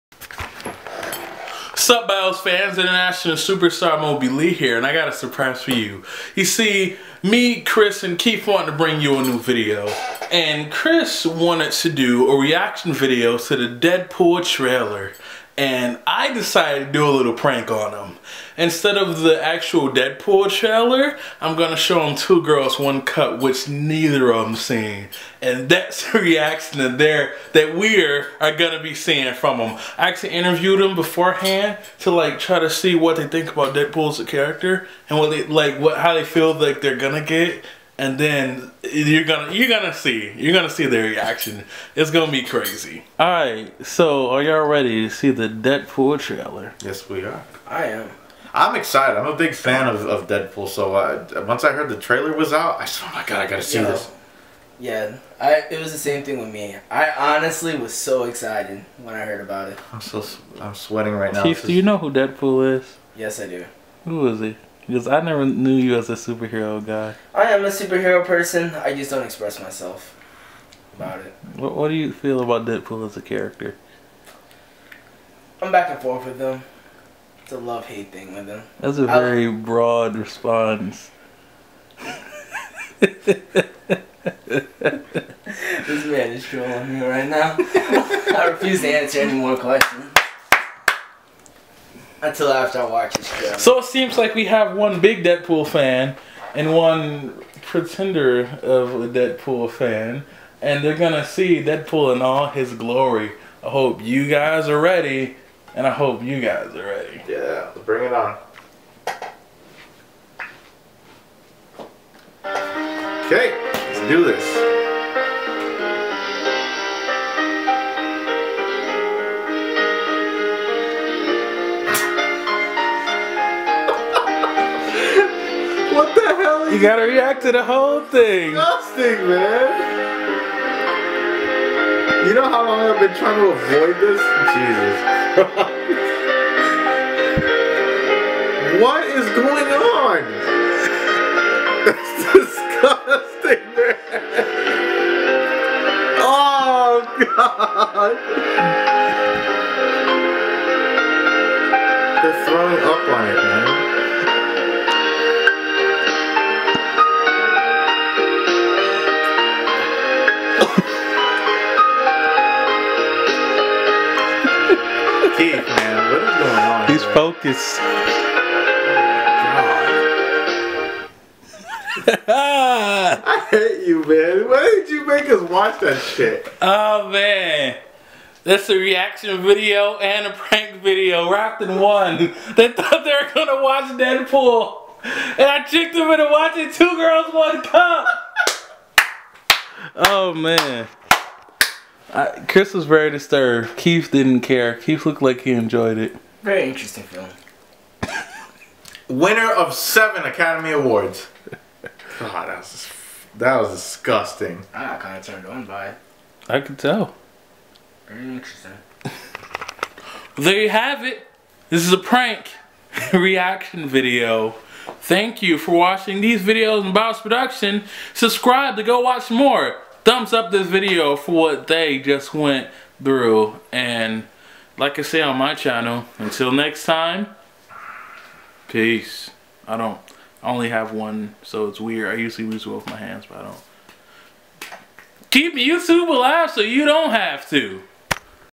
Deadpool Trailer Reaction Prank
Chris and Keith think they will be watching the new Deadpool Trailer  but they are in for a rude awakening like never before.